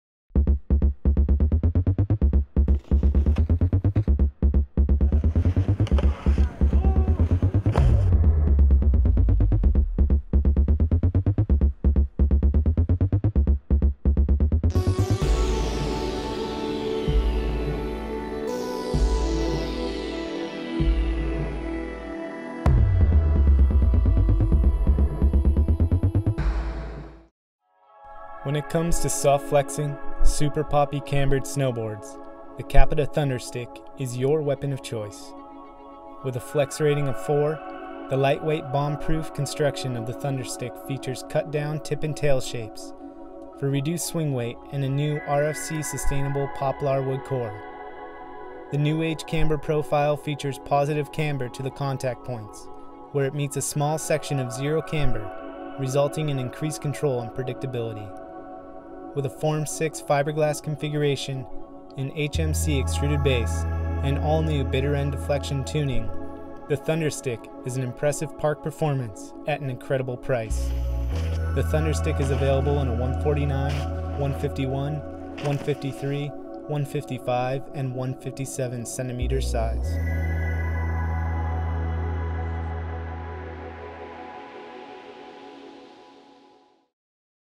CAPiTA Thunderstick 2015/2016
When it comes to soft-flexing super poppy cambered snowboards, the CAPiTA Thunderstick is your weapon of choice.